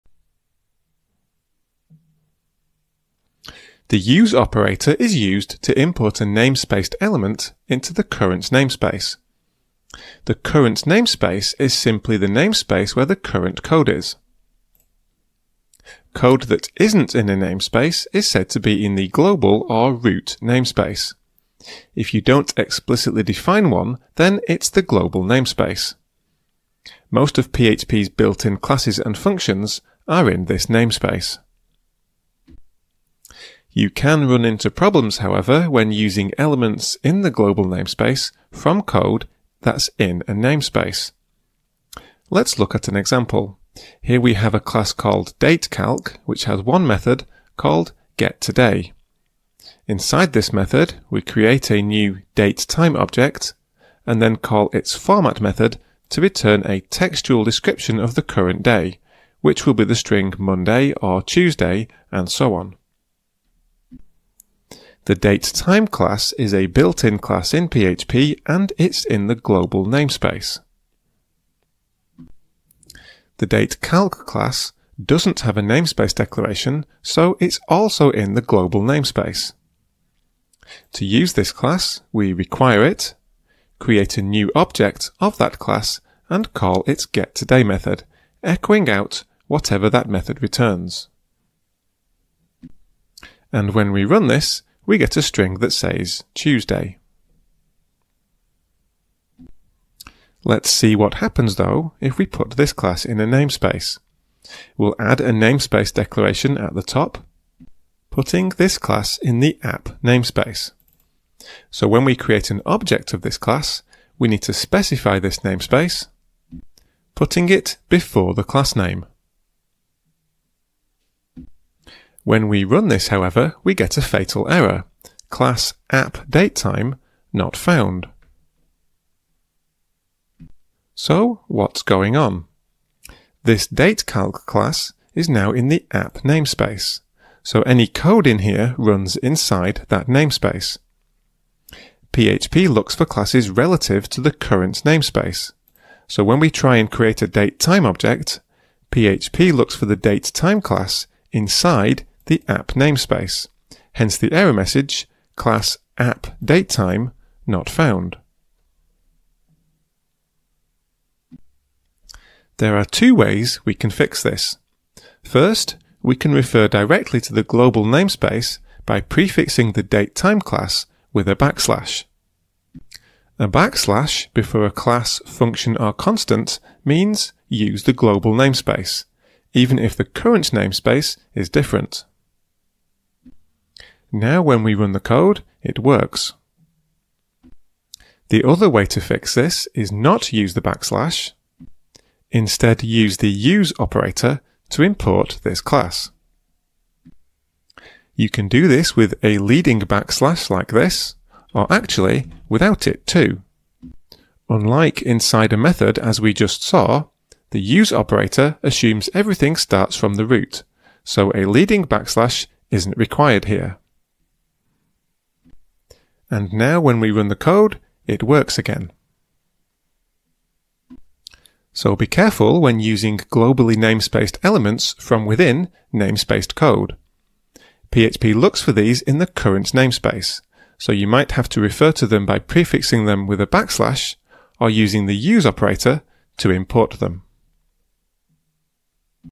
PHP namespaces 7/10: Relativity and the global namespace
We'll continue this mini-course on PHP namespaces with learning about the global namespace and how namespaces are related to each other.